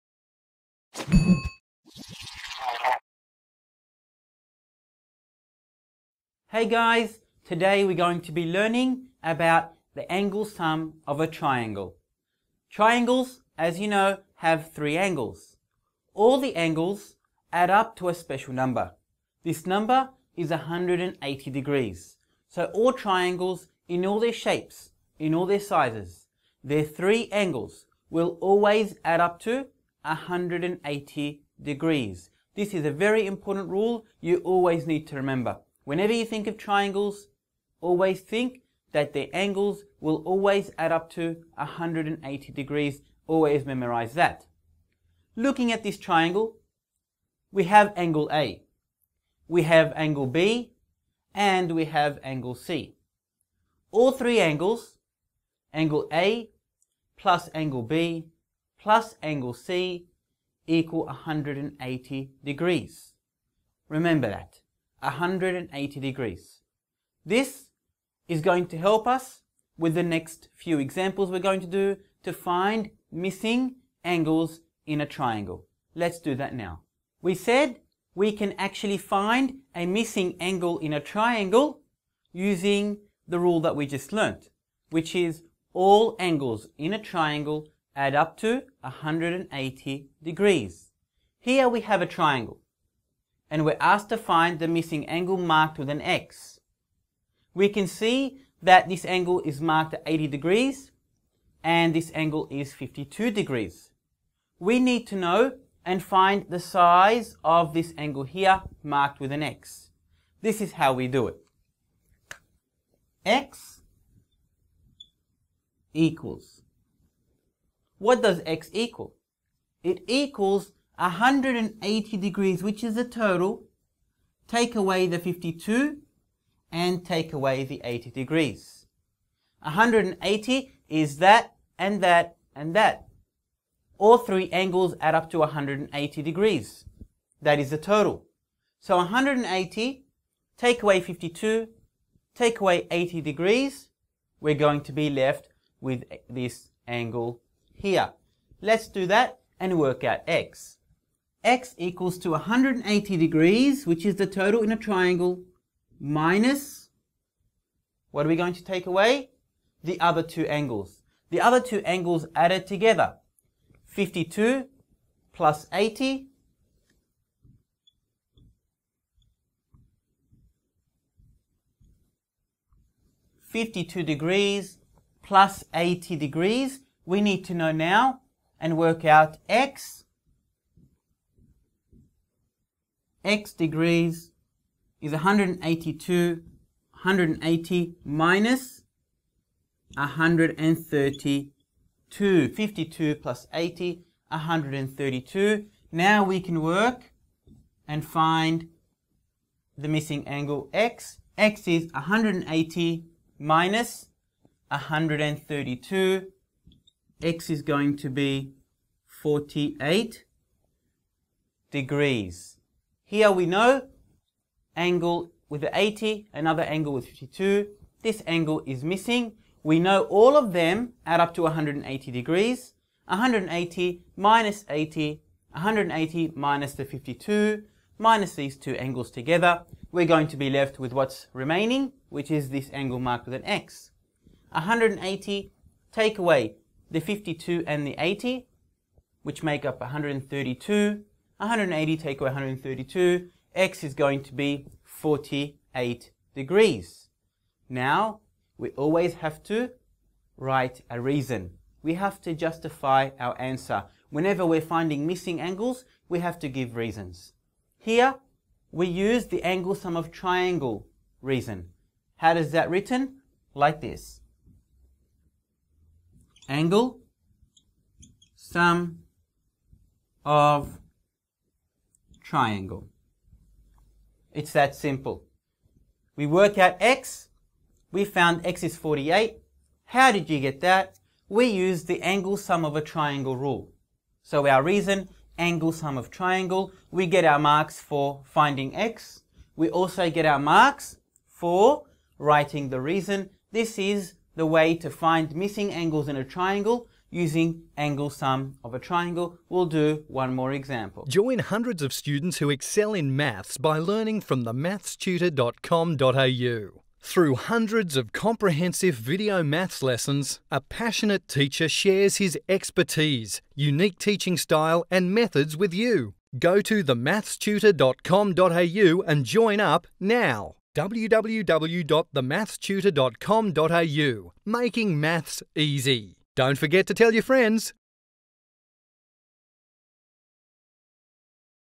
Maths Help Online - Angle sum of Triangle
Learn how to find a missing angle using the angle sum of a triangle rule.